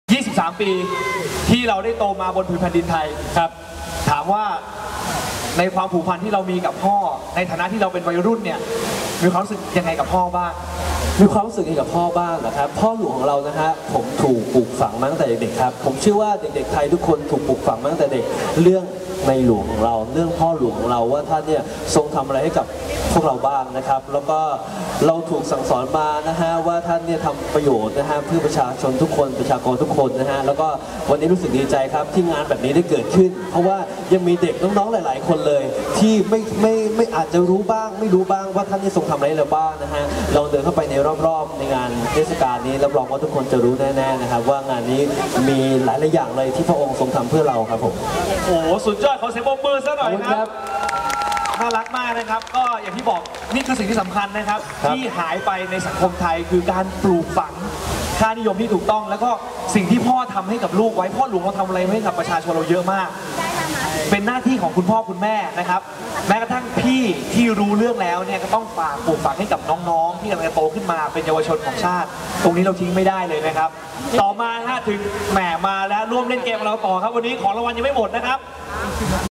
11032555 เตชินท์ พูดคุย 2
11/03/2555 งาน 84 พรรษา ประโยชน์สุขสู่ปวงประชา ณ ศูนย์การประชุมแห่งชาติสิริกิติ­์